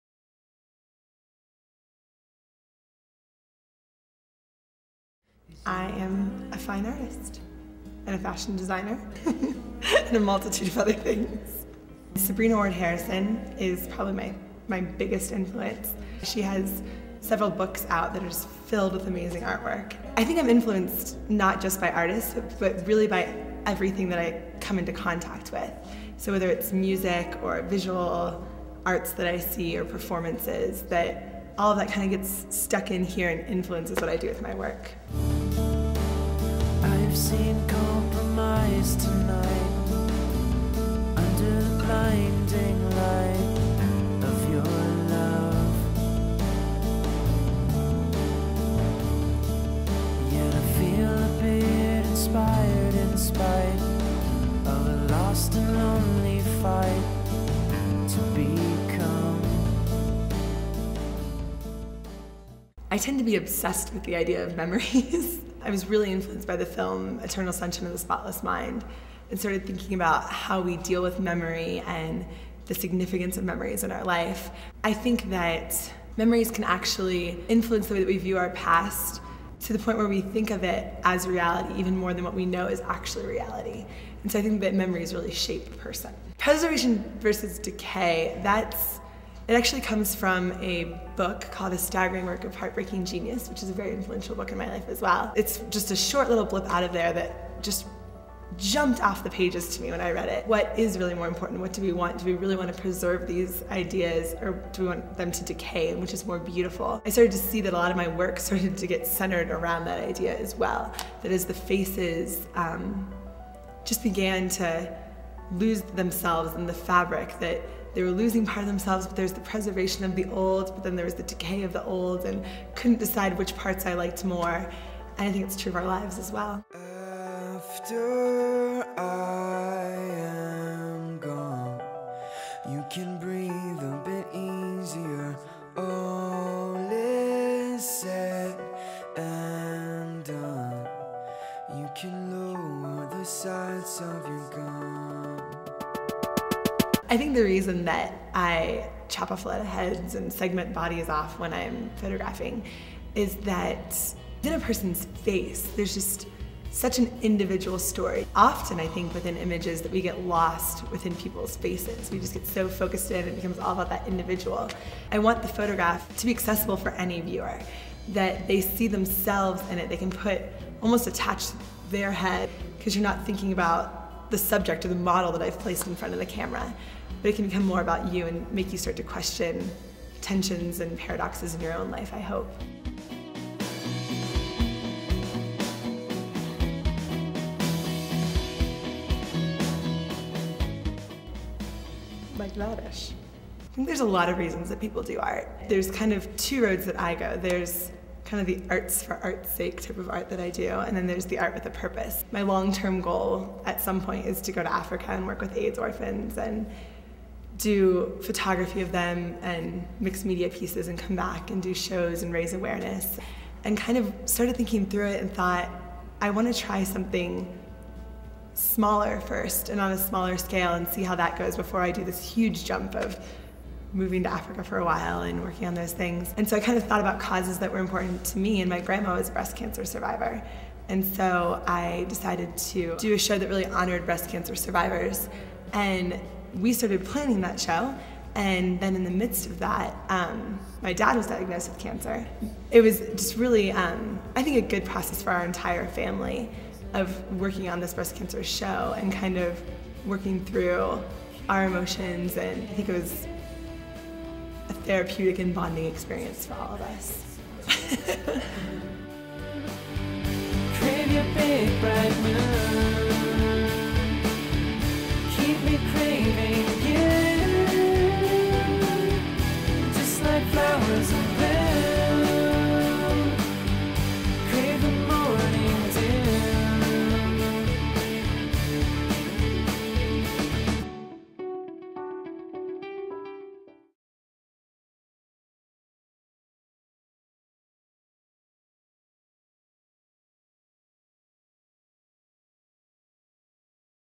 Out of Frame
We had the pleasure of doing this profile of Nicole McAdoo-Popovich, a Denver based mixed media photographer. We spent hours talking with her and digging through her work, struggling with how best to display her as a person and an artist. Our solution was to let our camerawork and editing emulate her artistic style, with the goal of letting her work permeate ours. We hope you like it.